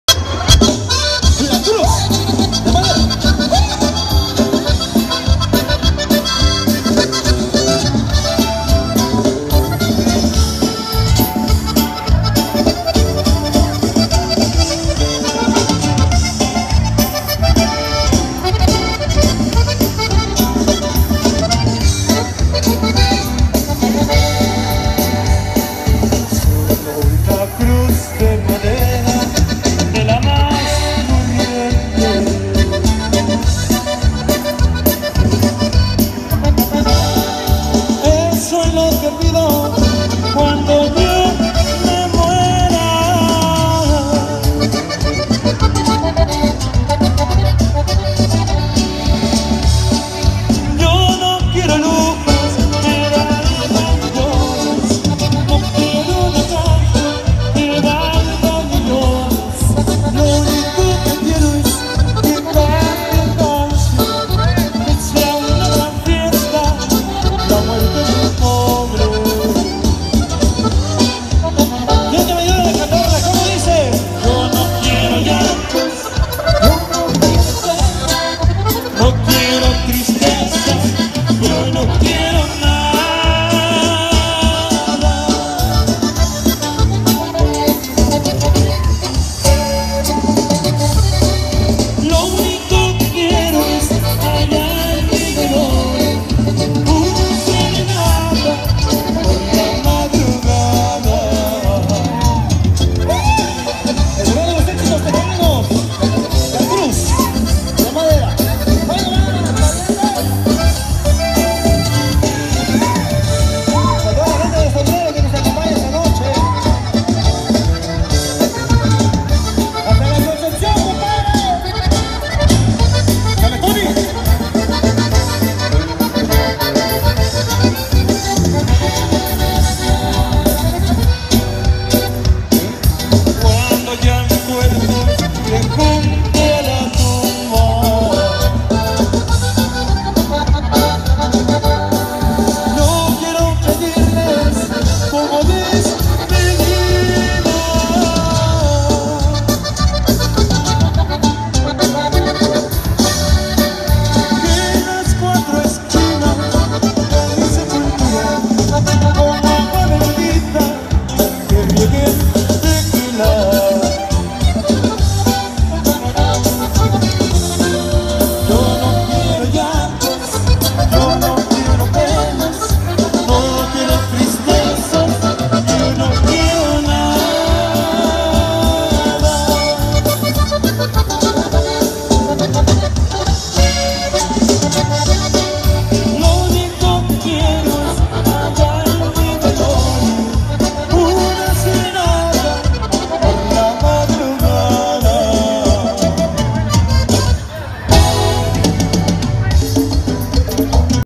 GRUPO FUEGO CRUZADO-ACAMBARO-2015
Grupo Fuego Cruzado en las Fiestas de Acámbaro Gto.